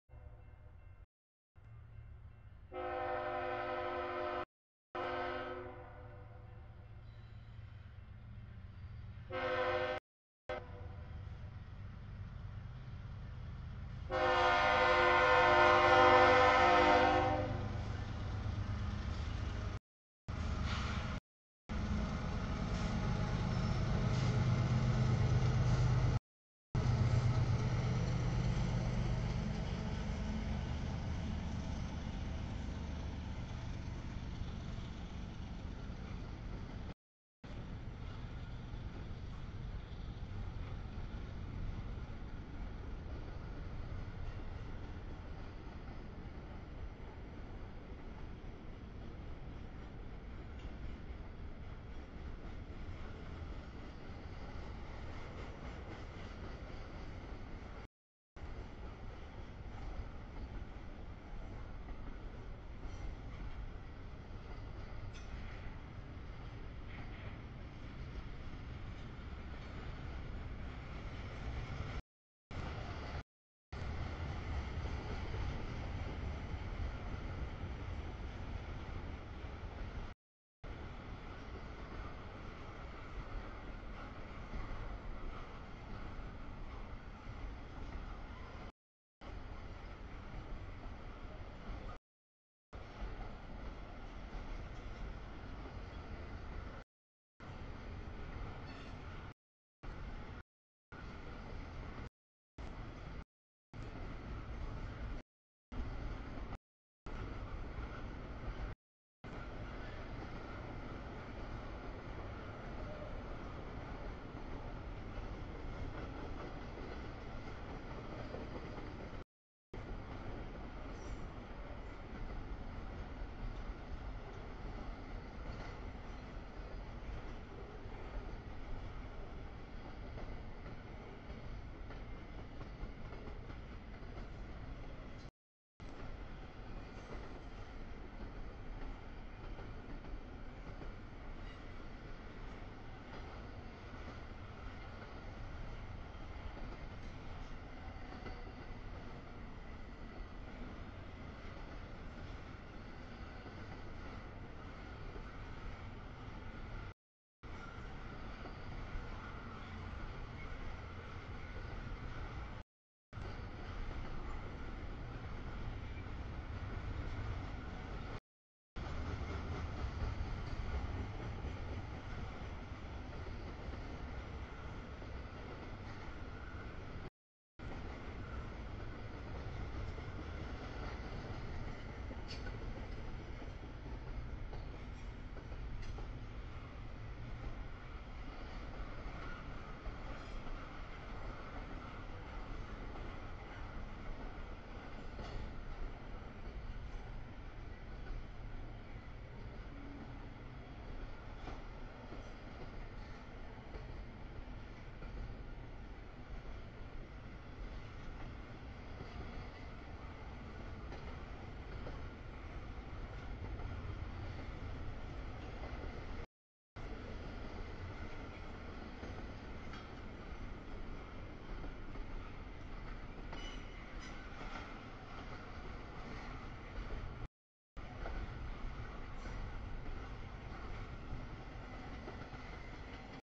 12- 10-2018 00:40 nb stack CSX Q167
00:40 nb stack CSX Q167
00:57 nb stack CSX 3147 Q157
01:12 nb stack trailers CSX Q009
02:18 nb stack CSX Q159
03:25 sb auto rack CSX Q262
03:56 sb stack CSX 3374 Q162
04:12 nb auto rack CSX Q261
05:22 nb stack CSX 3141 Q007
06:39 sb stack trailers CSX Q010
07:19 nb stack CSX 3236, 879 Q003
08:45 sb auto rack, mix CSX 5317, 5393, 983 Q403
08:57 nb tanks CSX 9017, 16 K683
09:47 sb stack trailers CSX 827, 837 Q008
10:34 sb stack CSX 3198, 5425 Q158